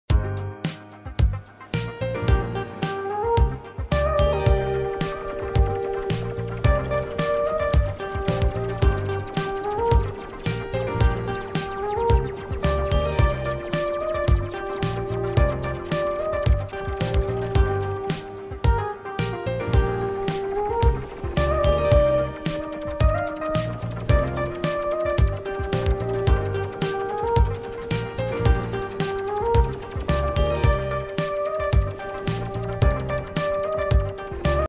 HFK-3000 waste Thermo bonded wadding opening machines for recycling soft comforter inner filling
We are direct machine factory and we have CE certificate for some machines.

comfort coiling packing machine
quilt rolling packing machine
quilting workshops quilt rolling packing machine